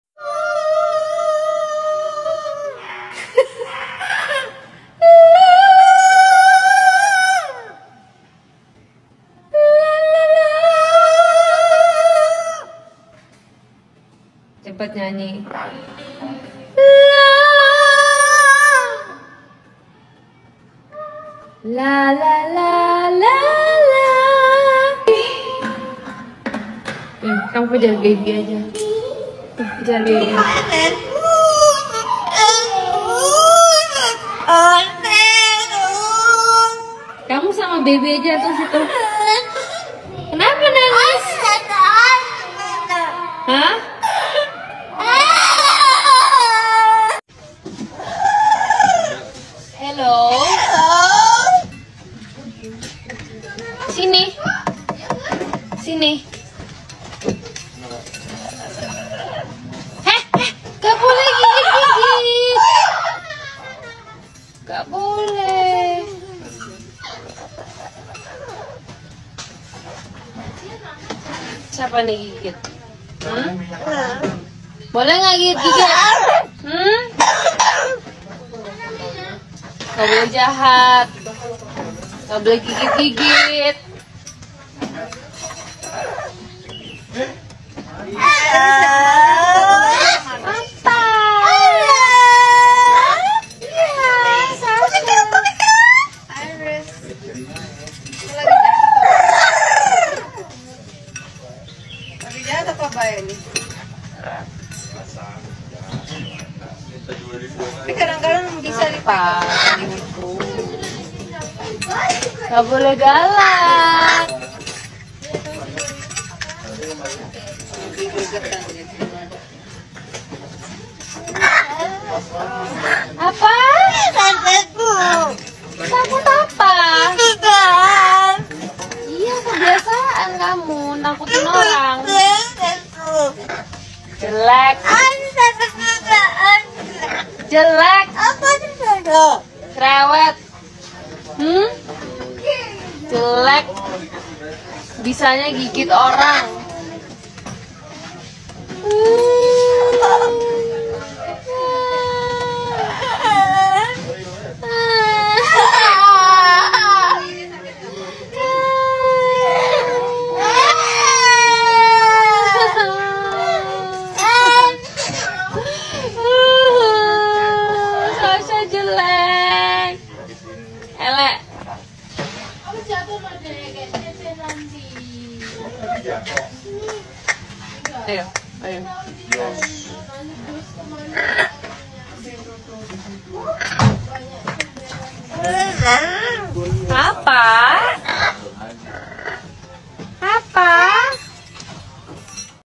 Burung Paruh Bengkok Pintar Nyanyi Dan Juga Ngomong
Burung Nuri
Suara Burung Nuri
Burung Nuri Kepala Hitam

Burung Parrot Ini Namanya Sasha Peliharaan Jacqueline Wijaya